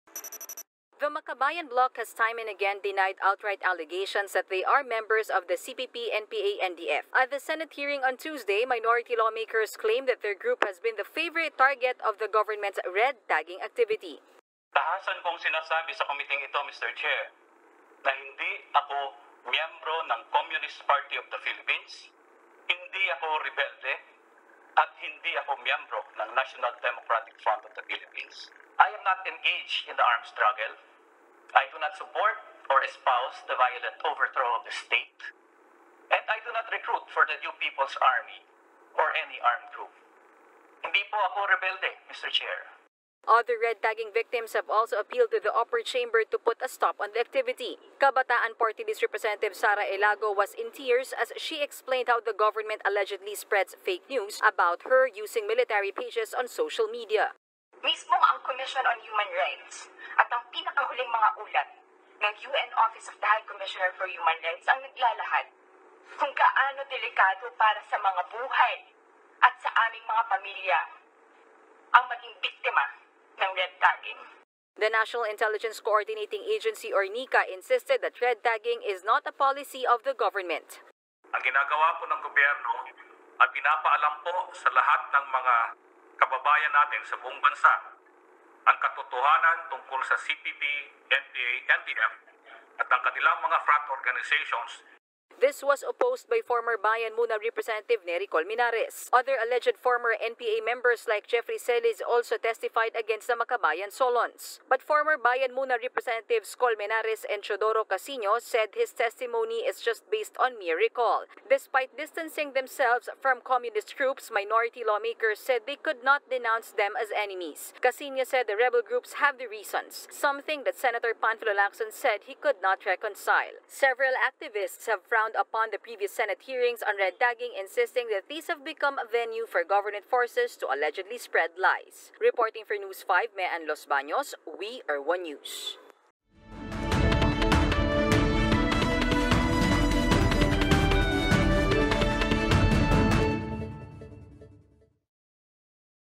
Makabayan bloc and security officials face off in Senate hearing
Minority House lawmakers appeal during the Senate hearing to stop the 'red-tagging.' Security officials, on the other hand, maintain that red-tagging isn't gov't policy to begin with.